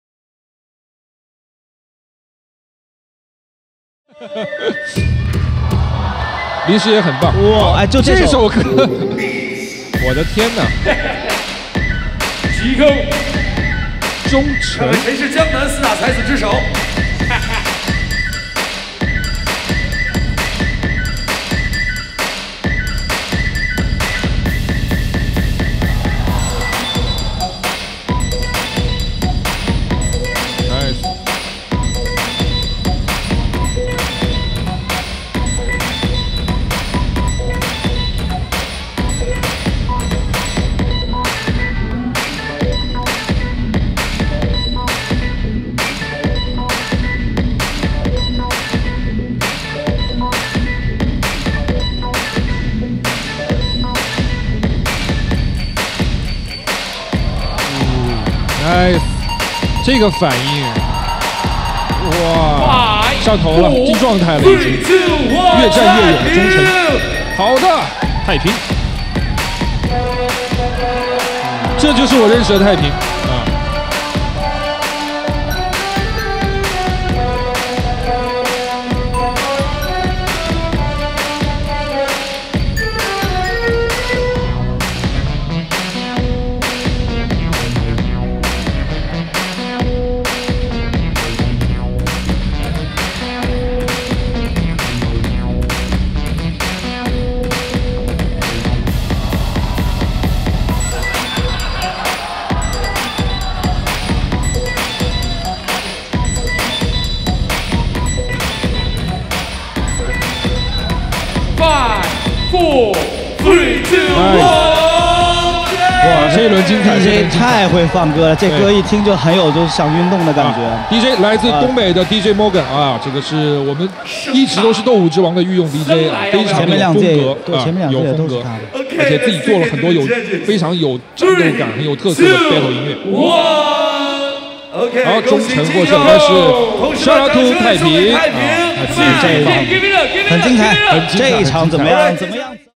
GCO vs 太平｜Best 8 @ WHO IS POPPING KING 2020｜ LB-PIX
WHO IS POPPING KING 2020
-TIME:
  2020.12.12

-JUDGE :
  SONIC (冯正)
  STONE (石头)
  JR.TACO (小海)

-DJ：
  MORGAN BEATZ

-MC:
 KILLA4NIA (鱼头)
 PINGPING (平平)

-BATTLE GUEST:
 TIRO, 钟晨, 飞龙, TRAIN, 王子川,
 林师爷, 陈斌, 孟飞, 鸡婆,
 太平, ROLZEE, 烟灰, 林溪,
 大发, ROCKIT, 余珊珊, BIG.D,
 小刀, 小飞, COLIN, SHARKBOMB,
 唐文, 硕仔,张浩然, 小五, 小宇, 王炎, BIBI